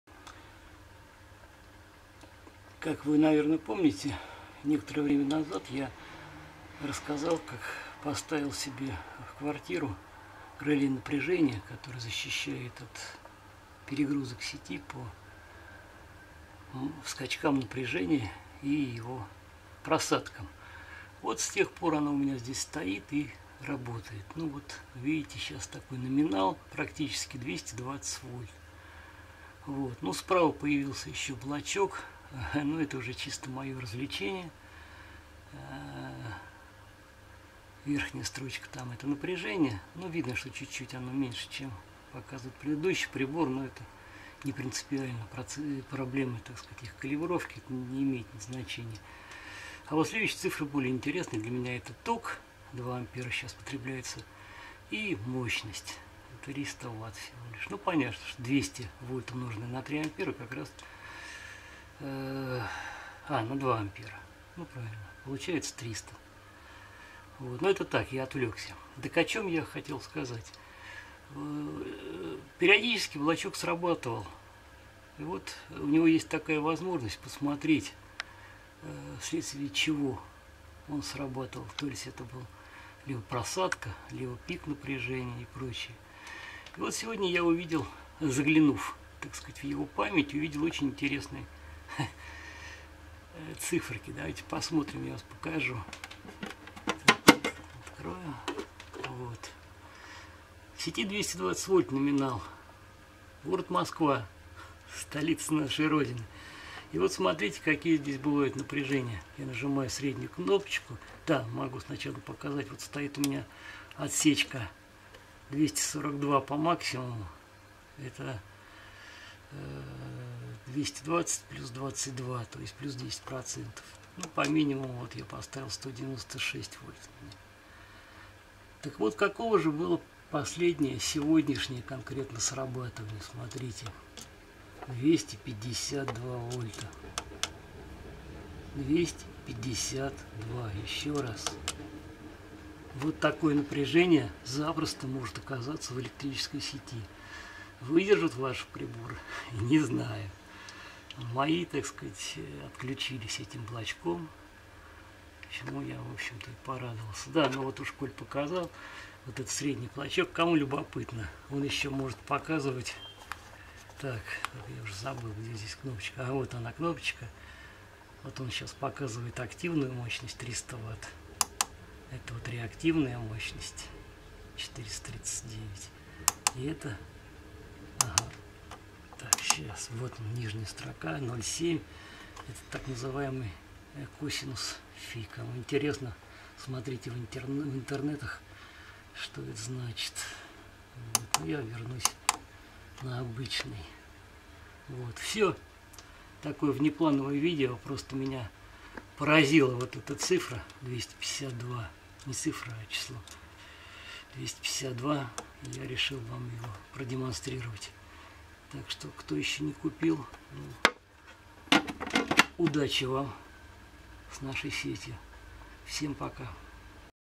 250 вольт в электросети? Легко!
Полгода назад поставил себе в квартиру защитное реле RBUZ, вот такие броски напряжения оказываются бывают в электросети.